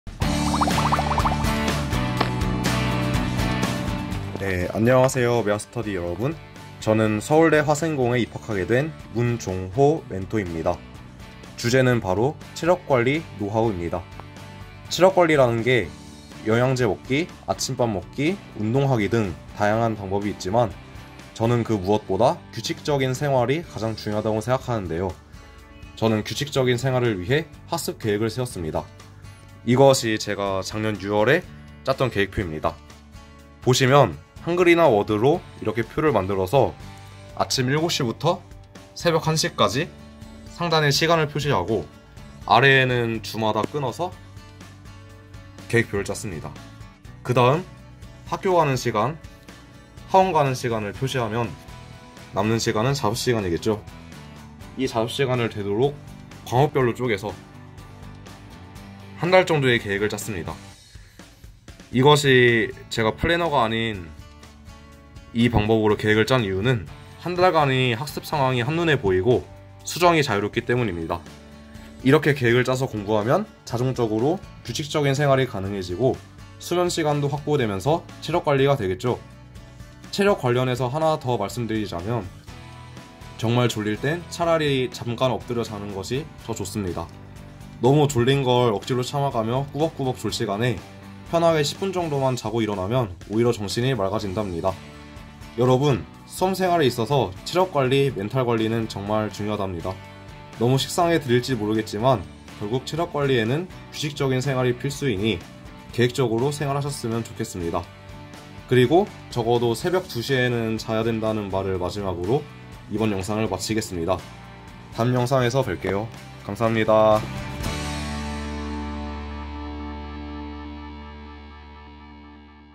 [메가스터디] 서울대 선배가 전하는 체력 & 멘탈 관리 꿀TIP
메가스터디 장학생 선배는
6월 학평을 어떻게 준비했을까?

서울대 선배가 실제 작년 6월에 작성했던
계획표를 중심으로 전하는 체력 & 멘탈 관리 꿀팁!
놓치면 손해!!!